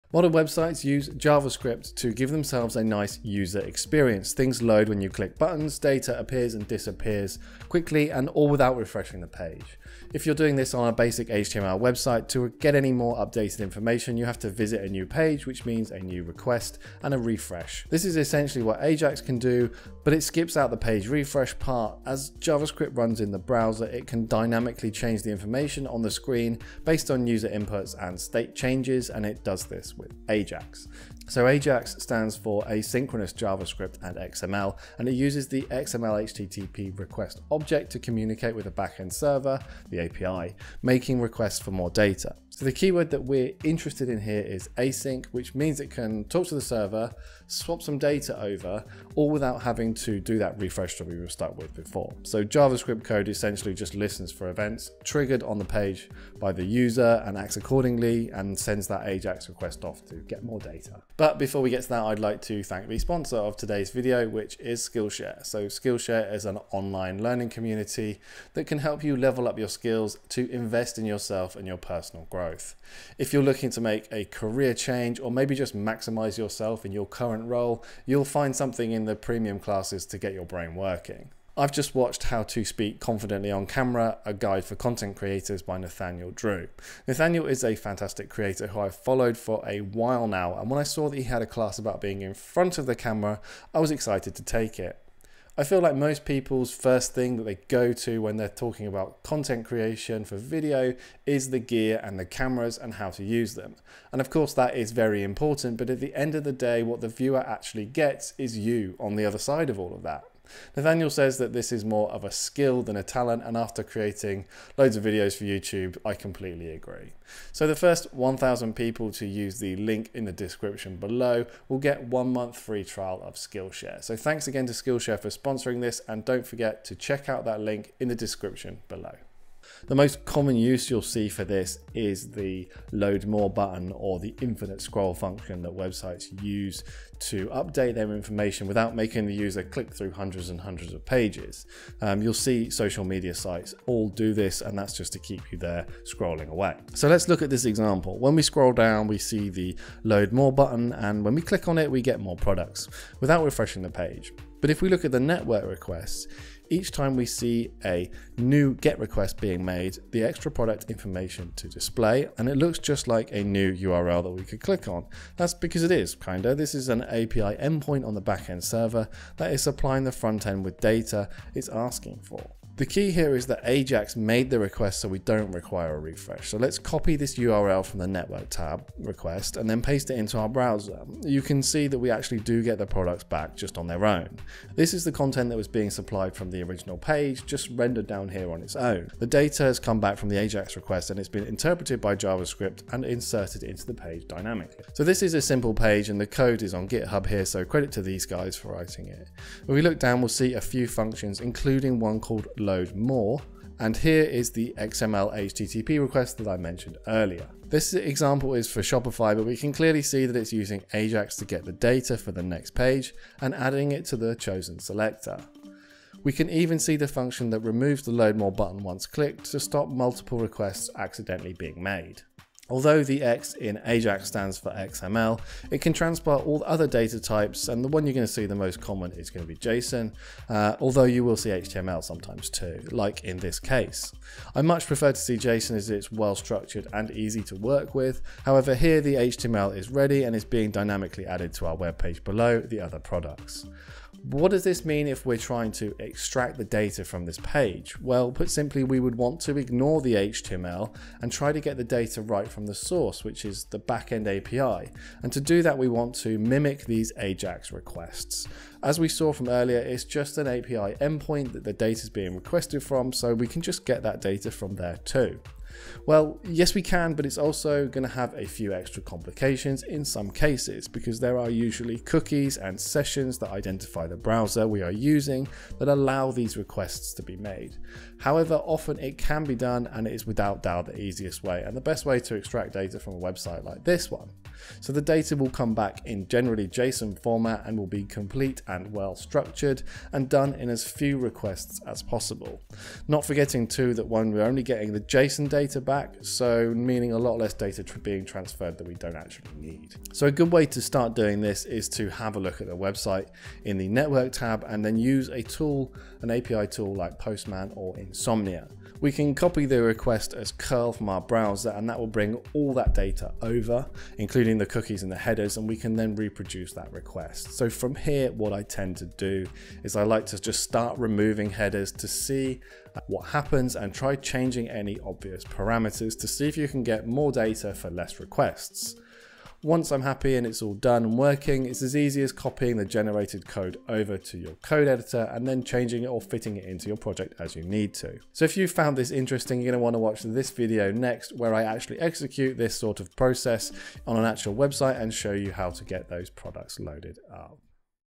How to get data from AJAX Requests in Python
AJAX is a commonly used tool by front end developers when writing JavaScript code. It fetches data from the backend system asynchronously meaning the website can be much more dynamic and allows new content to be loaded without having to refresh the page.

For those of you who are learning to extract data from sites knowing the basics of how this works will help you save time working out where the data is coming from and the best ways to get it out.